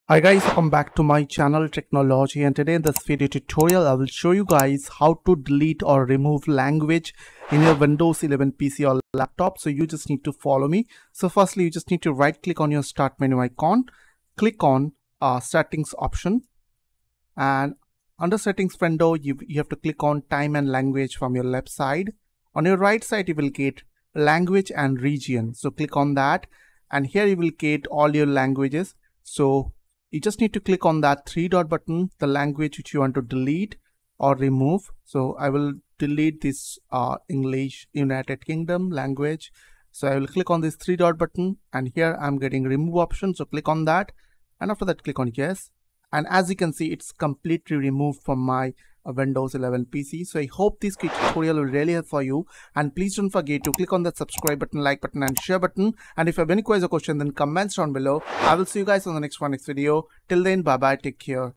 How To Delete or Remove Language in Windows 11
Here in this video tutorial, I will show you guys how to delete or remove keyboard language in your Windows 11 PC or Laptop.